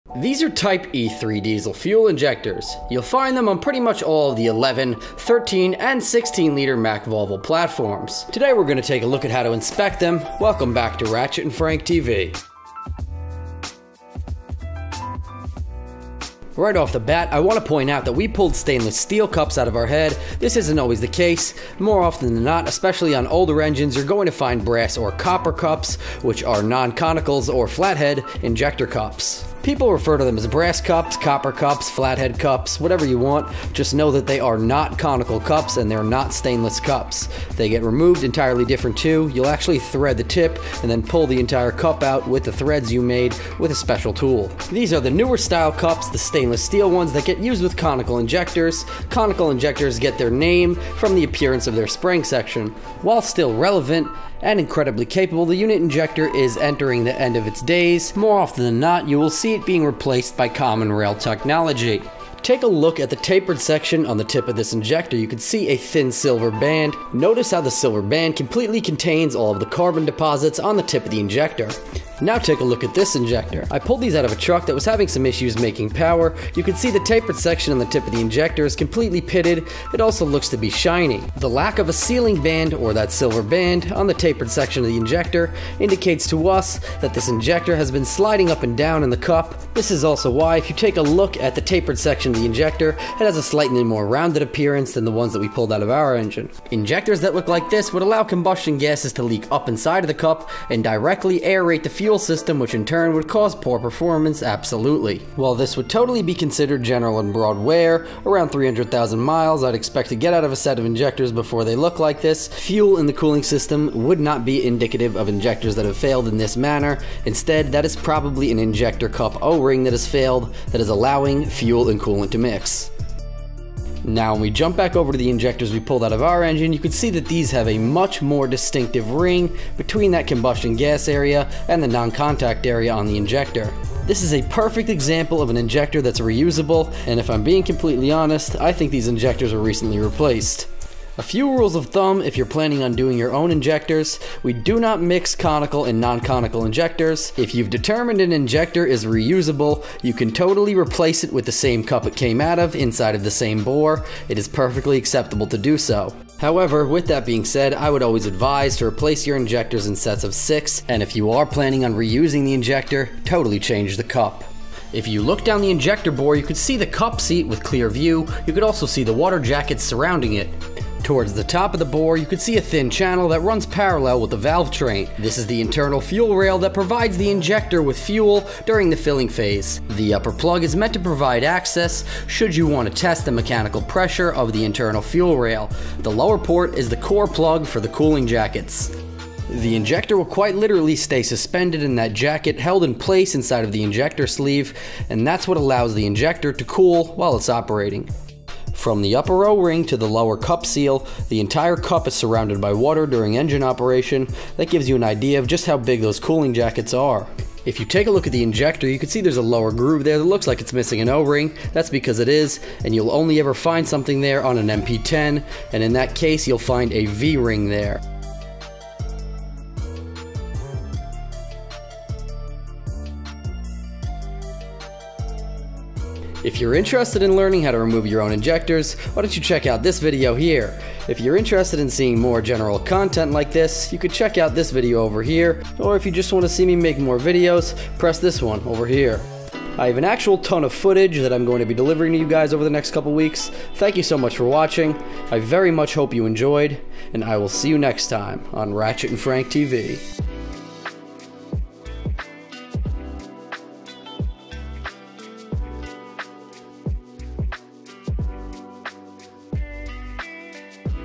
Everything you want to know about Mack/Volvo Diesel Fuel Injectors! [4K]
Sorry if I sound a little funny . a bit sick right now . I hope you guys enjoyed this video .

The video goes into detail on inspecting the fuel injectors that are most common inside of the Mack Volvo MP/D series engines. I also showcase some fantastic footage of failed injectors compared to reusable injectors. We also cover the difference between a failed cup seal against a failed injector o-ring